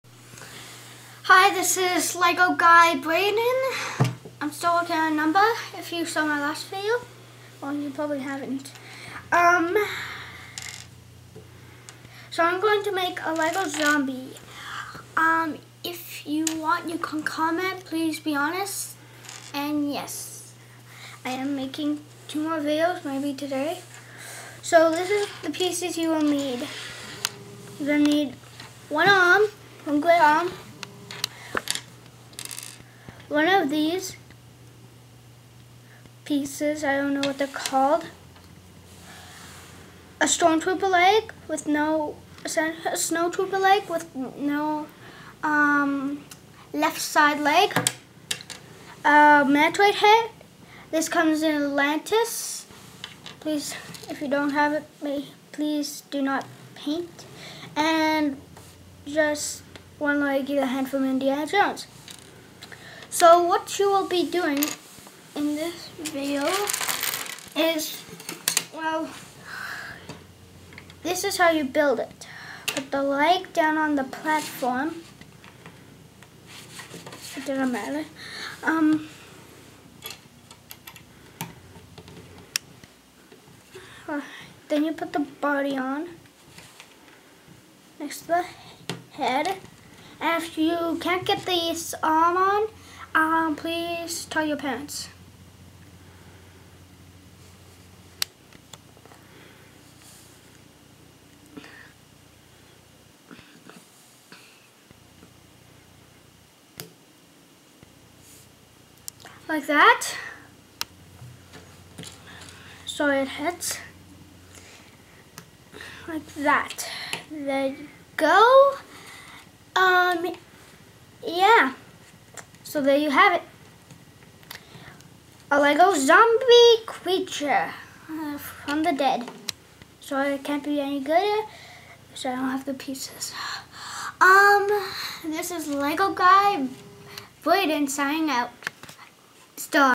How to Build a Lego Zombie Creature
Lego Man Braden shows us how to make your very own Zombie Creature from various Lego pieces.